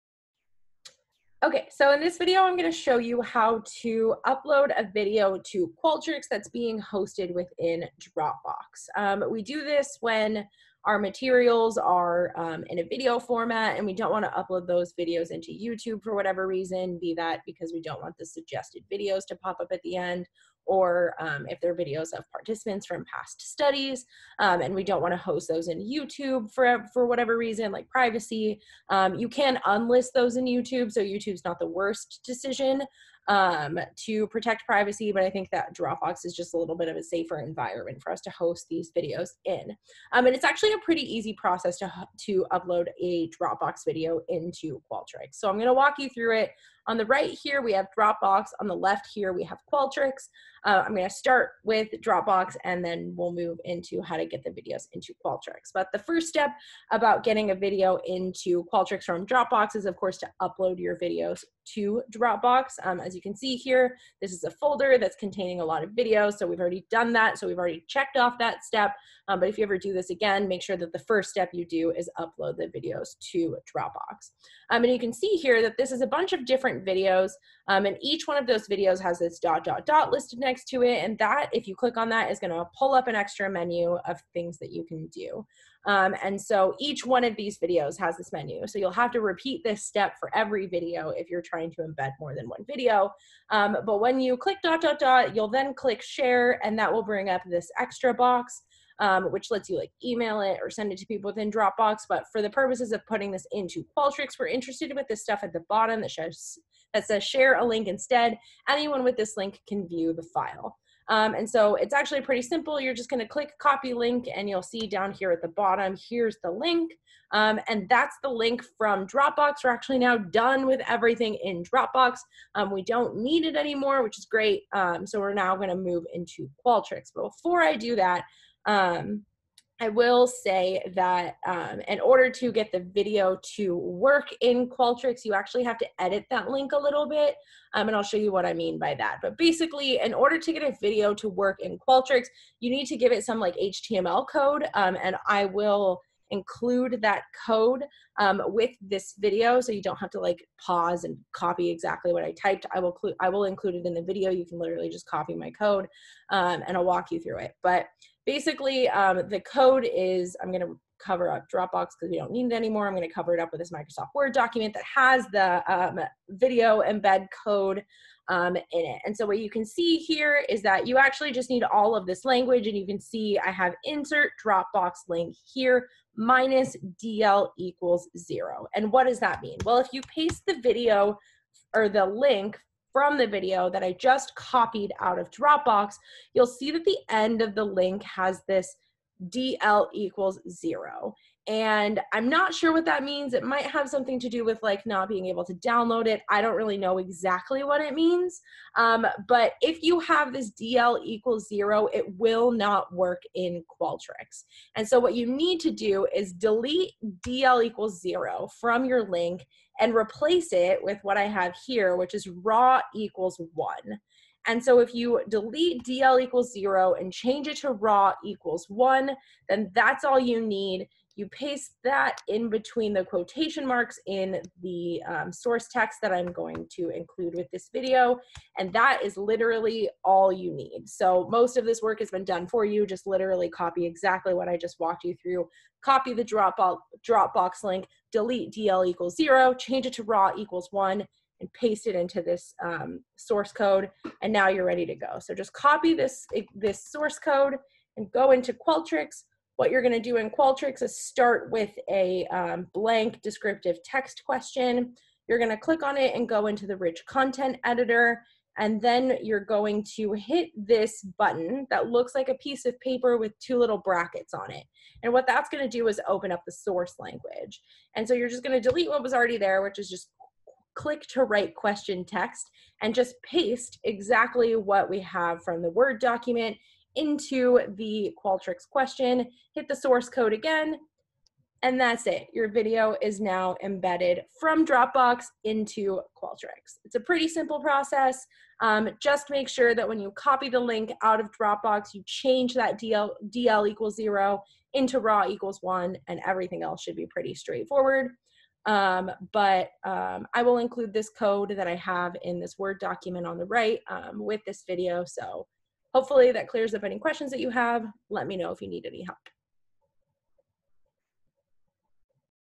Embedding Dropbox Video Into Qualtrics
Here's an explanation of how to embed a dropbox video into Qualtrics.

The video source code is below. The Youtube description box won't let me include the alligator-shaped brackets so make sure to replace those when using the code:

[PUT CLOSE BRACKET HERE]